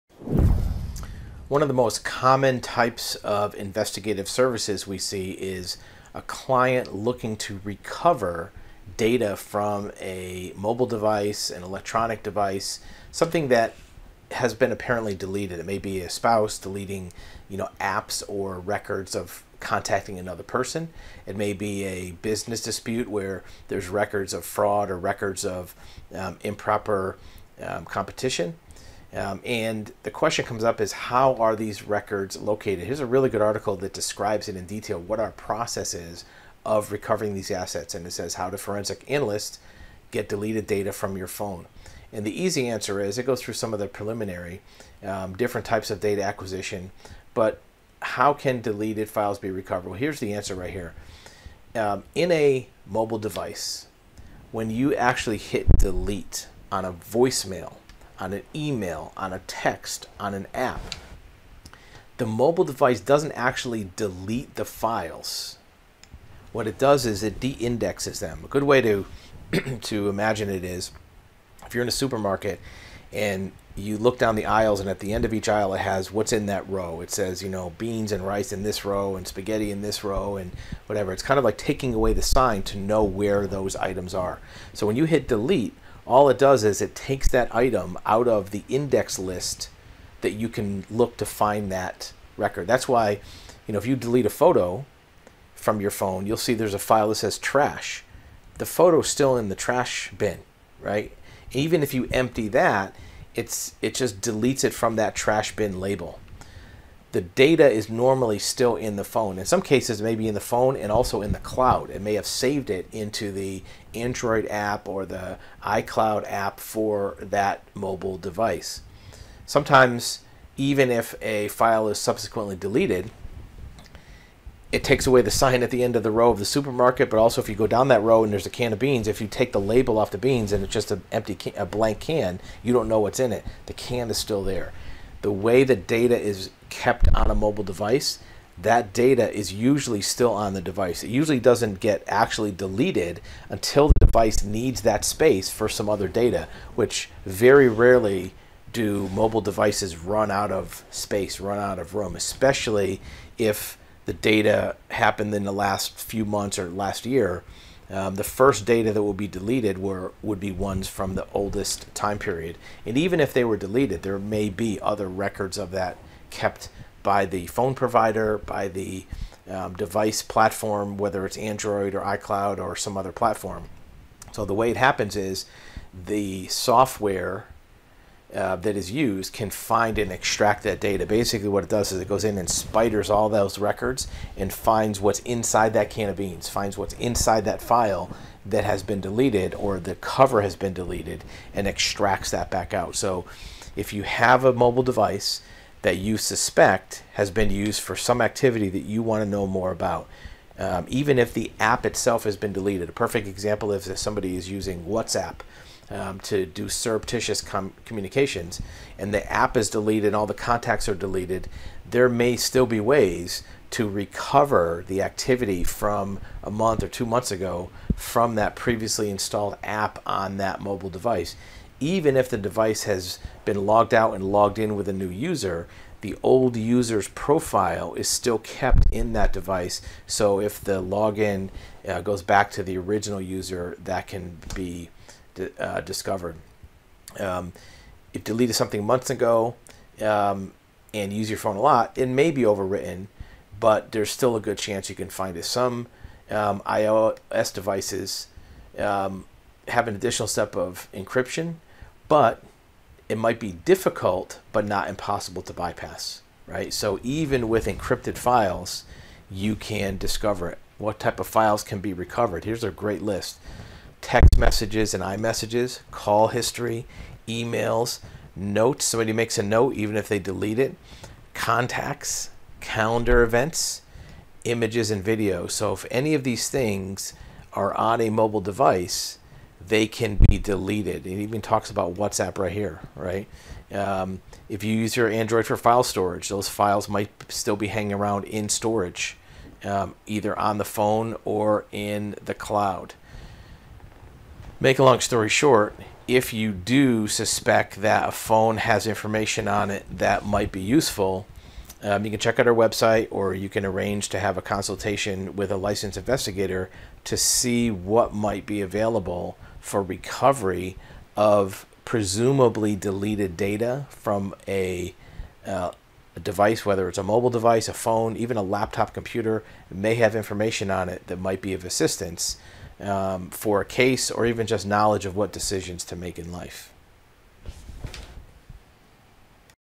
How to undelete hidden data from a mobile device
Text messages, photos, map locations, emails, documents, and app records are not completely erased even when deleted. Data recovery tools can restore the records for use in court or for proof of relationships, assets, and activities.